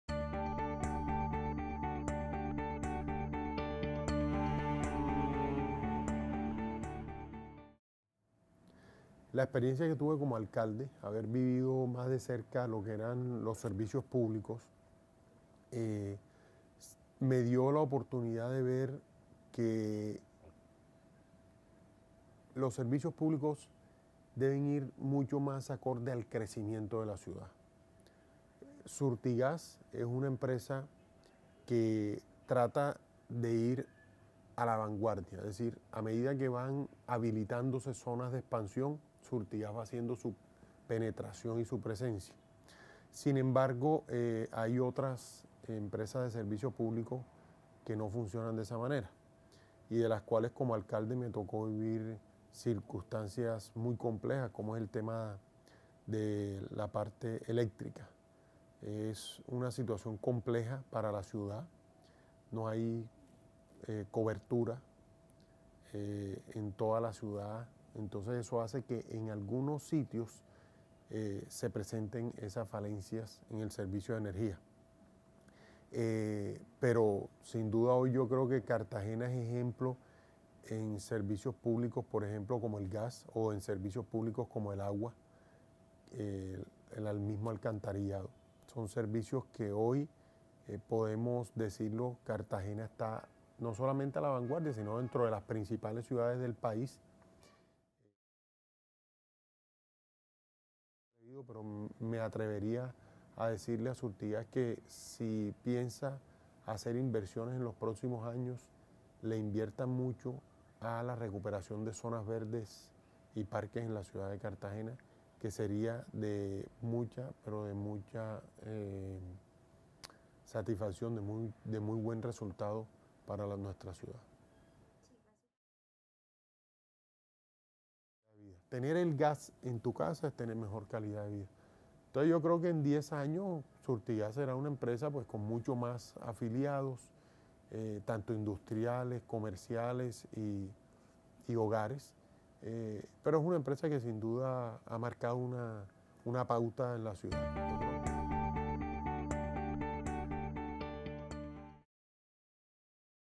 RepSurtigas15 - Exalcalde Dionisio Vélez
Para evaluar el impacto de Surtigas en su entorno de influencia se realizó una consulta al exalcalde de Cartagena (período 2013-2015) para el Informe de Sostenibilidad 2015.